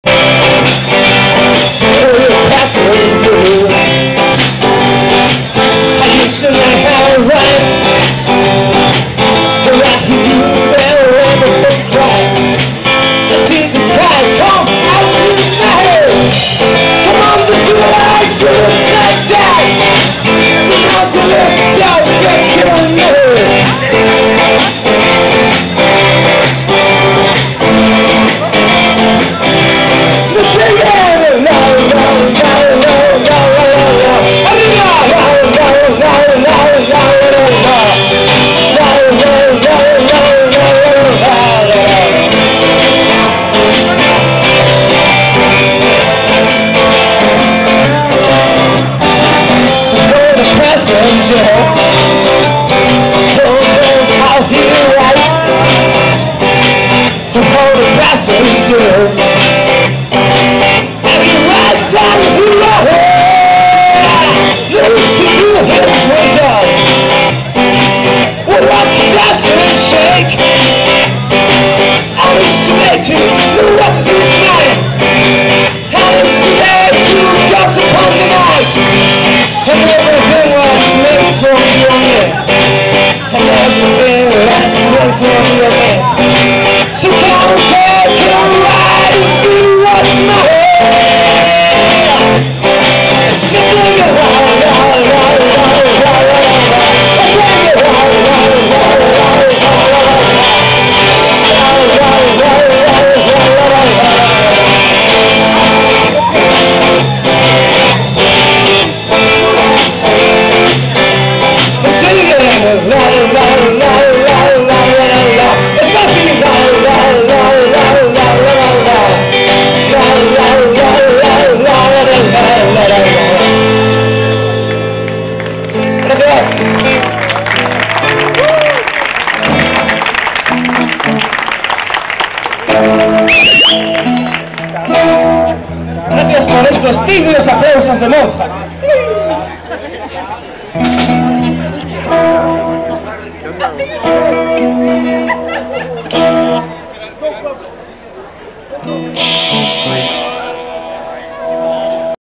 The Padron Peppers en acampadaxixon
Concierto solidario de The Padron Peppers en acampadaxixon acampadagijon el 28 de Mayo por la tarde